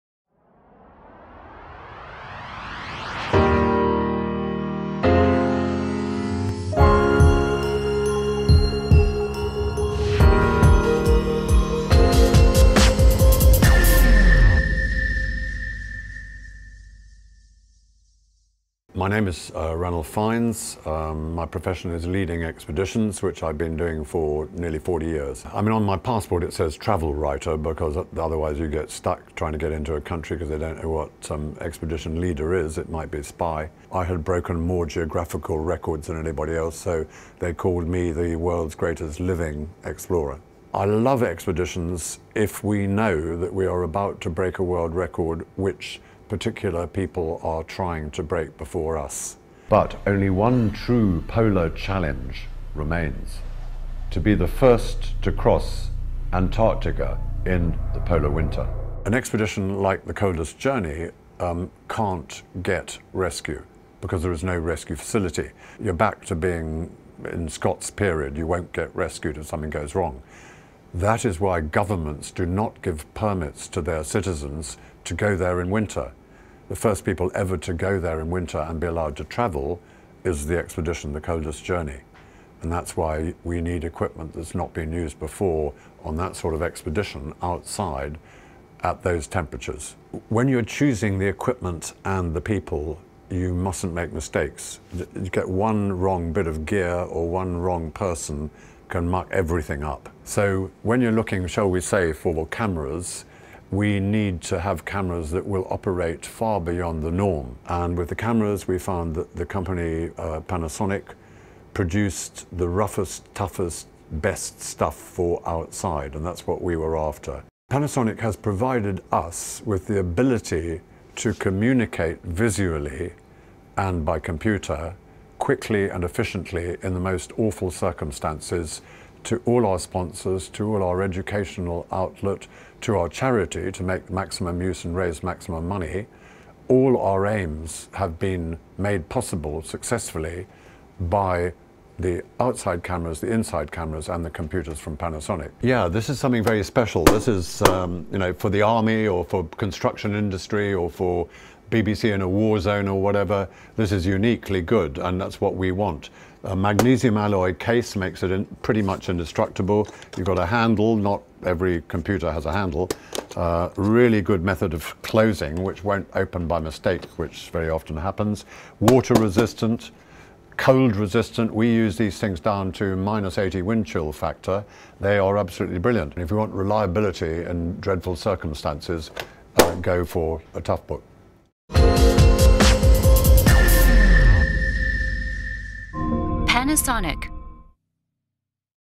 Greatest living explorer Ranulph Fiennes and the High Tech preparation for 'The Coldest Journey'
This video was originally published on Sep 6, 2013 by Panasonic Live@IFA 2013.

Meet Sir Ranulph Fiennes, the world famous explorer who relied on resilient all-weather devices from Panasonic to aid his latest Antarctic mission. Get inspired.

MEET SIR RANULPH FIENNES

It's no easy feat to be named the greatest living explorer by the Guinness Book of World Records, but British explorer and multiple record holder Sir Ranulph Fiennes has never been impressed by doing things the easy way. After both successfully scaling Mount Everest and crossing Antarctica on foot, Fiennes was looking for a new extreme challenge. He found it with the help of advanced expedition equipment from Panasonic.

HIGH TECH MEETS LOW MAINTENANCE

With cold-resistant equipment like our rugged Toughpad and photo and video cameras with long lasting batteries, the Panasonic line of high tech hardware was a perfect match for Fiennes and his team. Documenting the risky journey required reliable and durable equipment that always delivered accurate results.